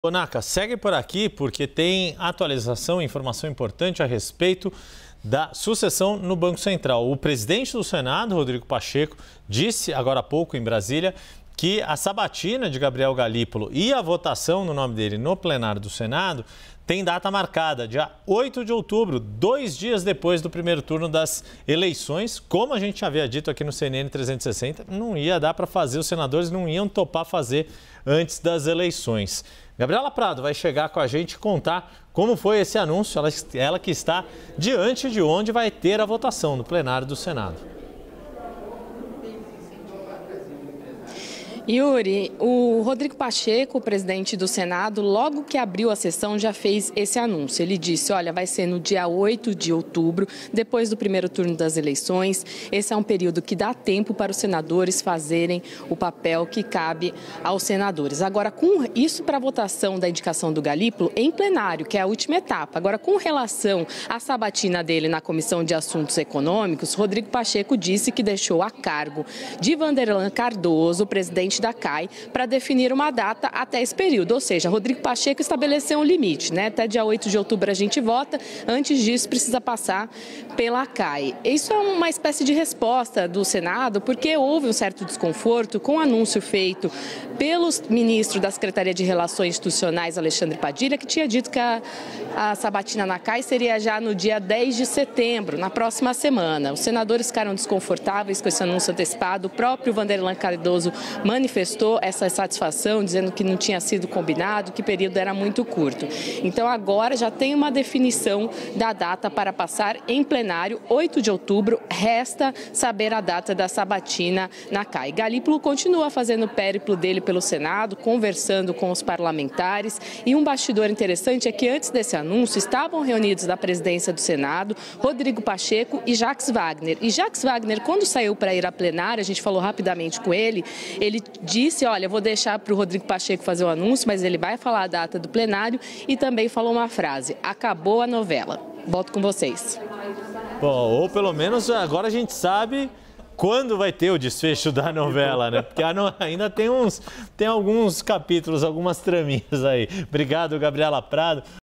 Sabatina de Galípolo no Senado será em 8 de outubro | CNN 360º
O presidente do Senado, Rodrigo Pacheco (PSD-MG), afirmou nesta quarta-feira (4) que a sabatina de Gabriel Galípolo, indicado ao comando do Banco Central, no Senado acontecerá no dia 8 de outubro, depois do primeiro turno das eleições.